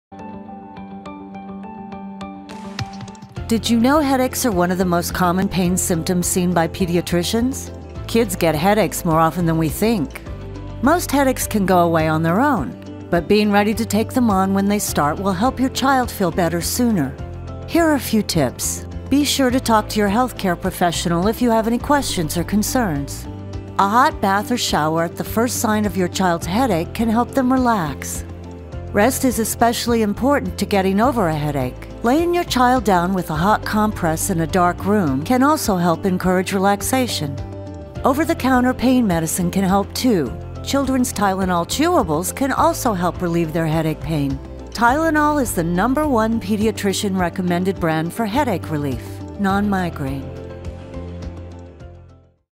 Top 3 Child Headache Treatments | HEALTHY ESSENTIALS®
Headaches in kids are one of the most common symptoms seen by pediatricians. Often, they will go away on their own, but there are steps you can take if your child complains of a headache to help them feel relief sooner. Watch this video to learn more about the top 3 ways to treat child headaches.